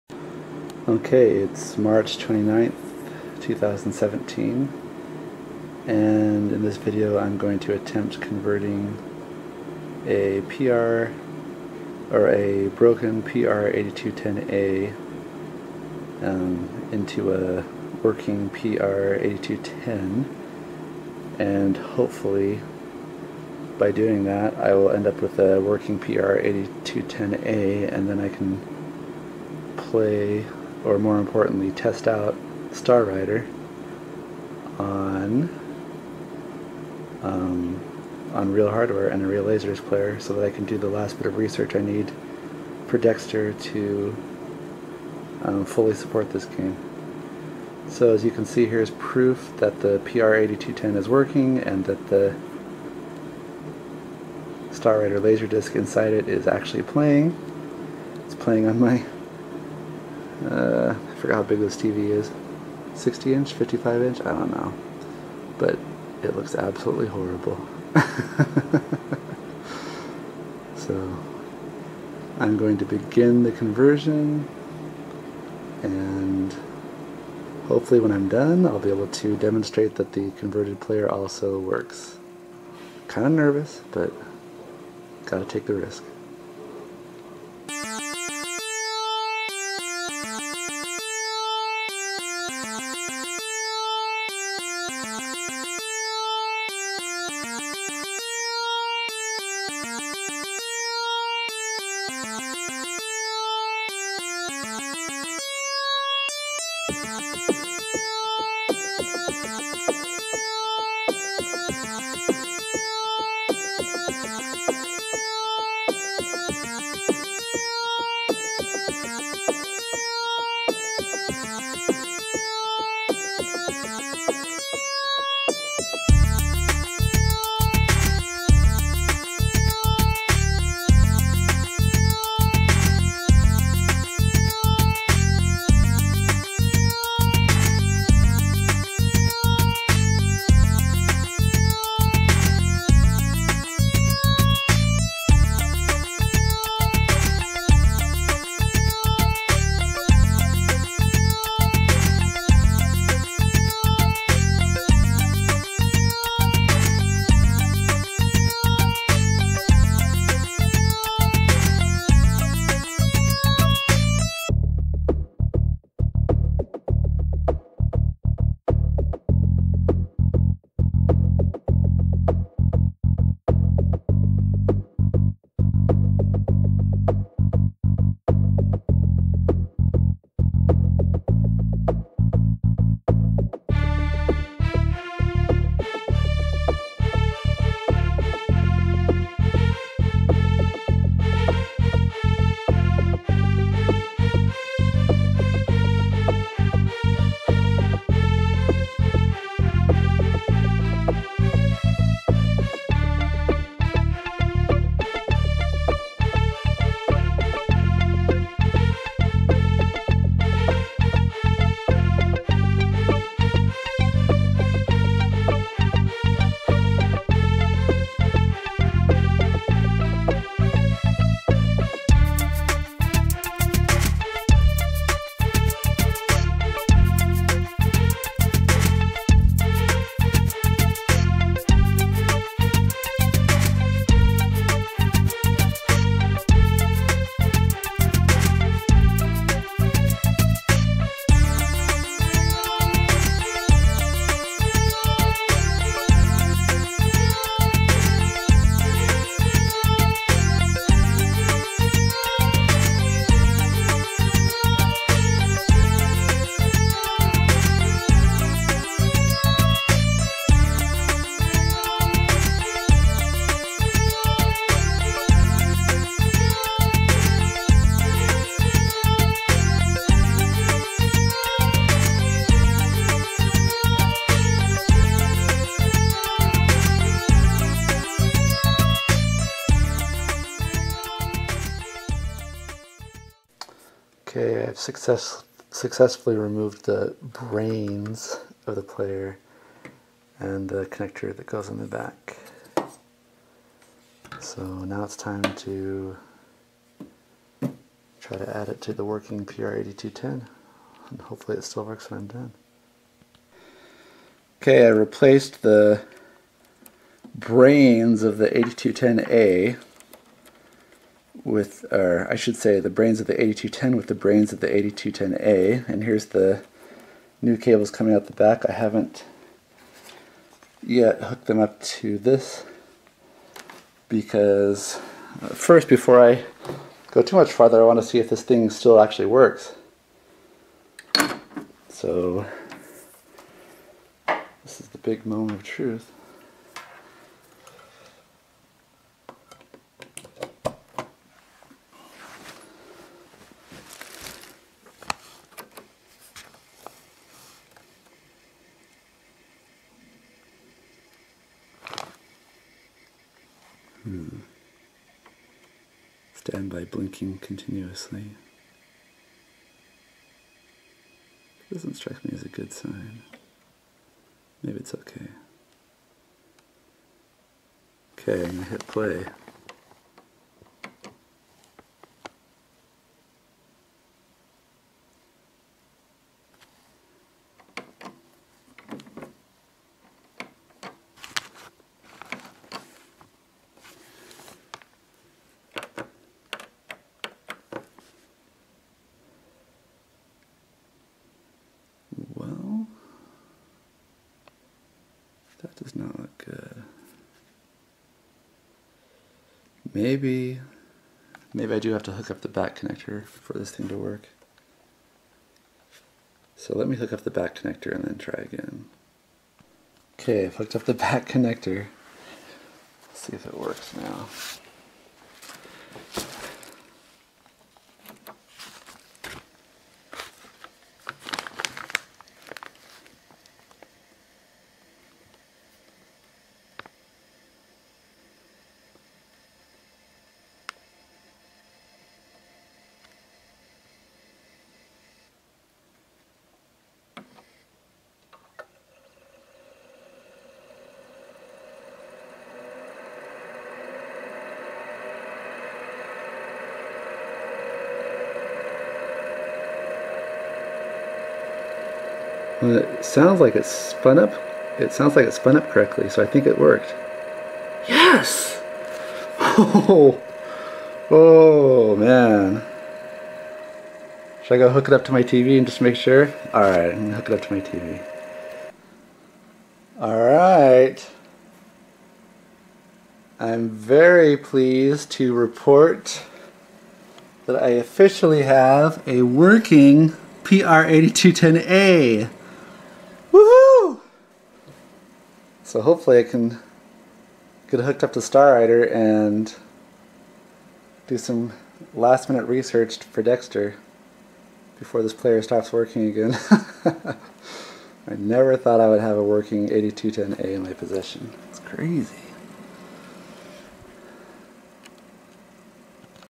Struggling with Star Rider : Resurrecting dead Pioneer PR-8210A laserdisc player
The Pioneer PR-8210A, very rare, is nearly impossible to find working.  However, the guts of a dead PR-8210A can be moved into a working PR-8210 which results in a hybrid working PR-8210/A player!  The laserdisc arcade game, Star Rider, uses this type of player.